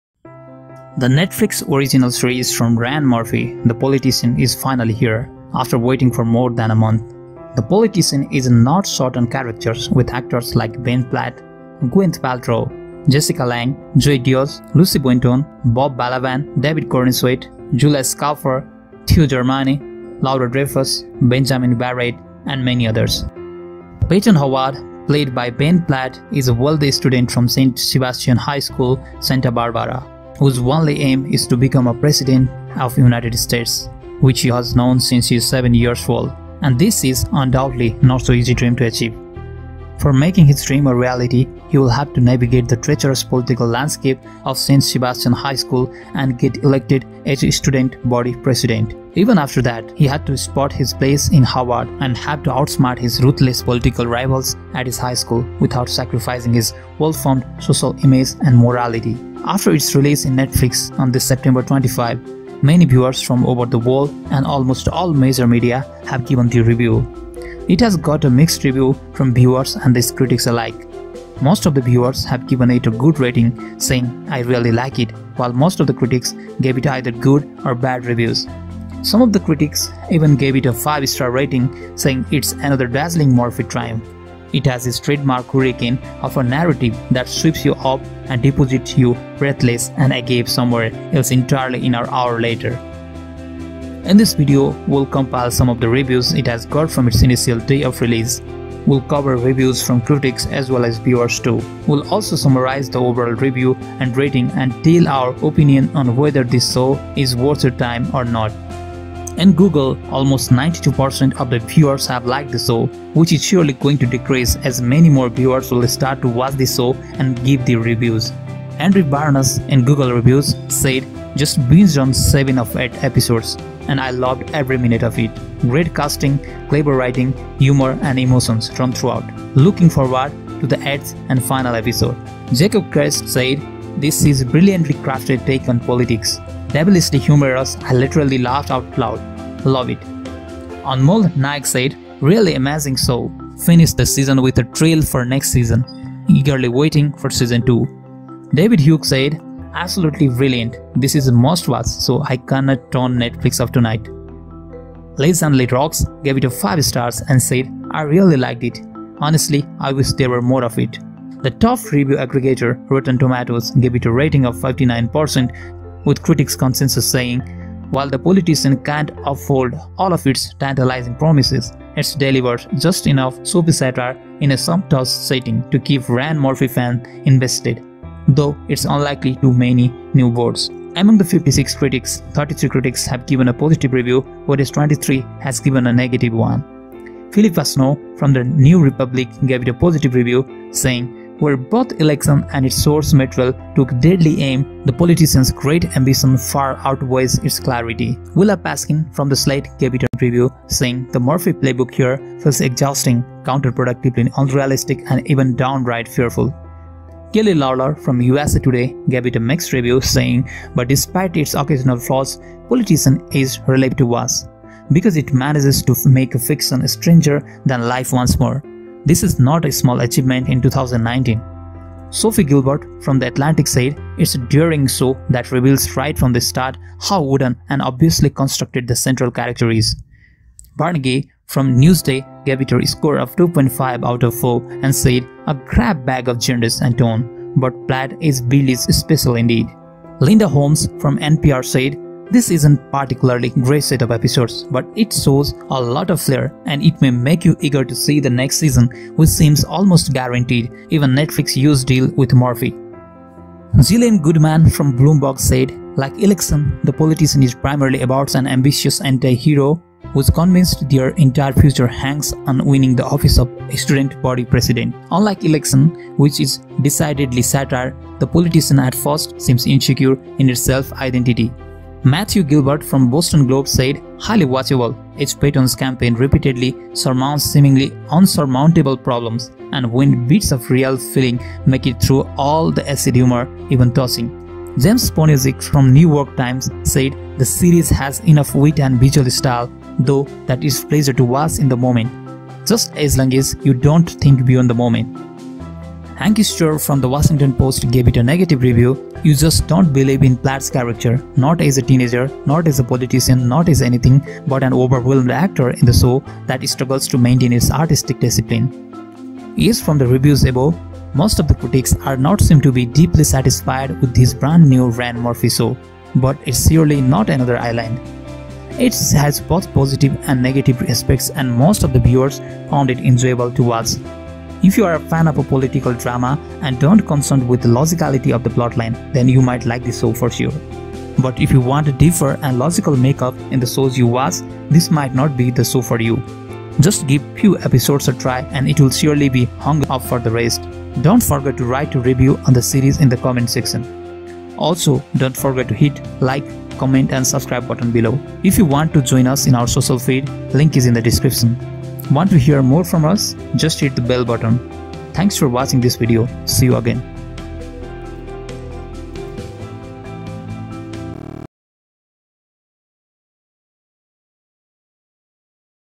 THE POLITICIAN NETFLIX REVIEW IS HERE !!!
Here we are covering The Politician review which just released on 27th September.
The Netflix Original Series from Ryan Murphy - The politician is finally here after waiting for more than a month.
 The politician is not short on characters with actors like Ben Platt, Gwyneth Paltrow, Jessica Lange, Zoey Deutch, Lucy Boynton, Bob Balaban, David Corenswet, Julia Schalepfer, Theo Germaine, Laura Dreyfuss, Benjamin Barett, and many others. Payton Hobart played by Ben Platt is a wealthy Student from Santa barbara California whose only aim is to become a president of the United States which he has known since he's 7 years old, and this is undoubtedly not so easy dream to achieve. For making his dream a reality he'll have to navigate the treacherous political landscape of Saint Sebastian high school and get elected as a student body president.
Even after that, he had to spot his place in Harvard and have to outsmart his ruthless political rivals at his high school without sacrificing his well-formed social image and morality.

After it's release in Netflix on this September 25, many viewers from over the world and almost all major media have given their review. It has got a mixed review from viewers and the critiques alike. Most of the viewers have given it a good rating saying "I really like it"
while most of the critiques gave it either good or bad reviews. Some of the critiques even gave it a 5-star rating saying "It's another dazzling Murphy triumph. It has his trademark hurricane of a narrative that sweeps you up and deposits you breathless and agape somewhere else entirely an hour later.".

In google, almost 92% of the viewers have liked the TV Show till now. Which is surely going to decrease as many more viewers will start to watch this show and give their reviews.
Henry Barnes said: Just binged on 7 of 8 Episodes (1 Season) and Loved every minute of it! Great casting. Clever writing. Humor & Emotions run throughout! Looking forward to the 8th (Final?) Episode!
Jacob Kerst said: This is a brilliantly crafted take on politics. Devilishly humourous. I literally laughed out loud. Love it!
Anmol Nayek Said: Really amazing show. Finished the season with a trail for next season. Eagerly waiting for season 2.
David Huke said : Absolutely brilliant, this a must watch .I so cannot turn Netflix off Tonight.?
Lizanne Le Roux gave it 5 stars and said : I really like it. Honestly, I wish there were more of it.

The Politician Netflix Review
The Politician Reviw
Netflix The Politician Review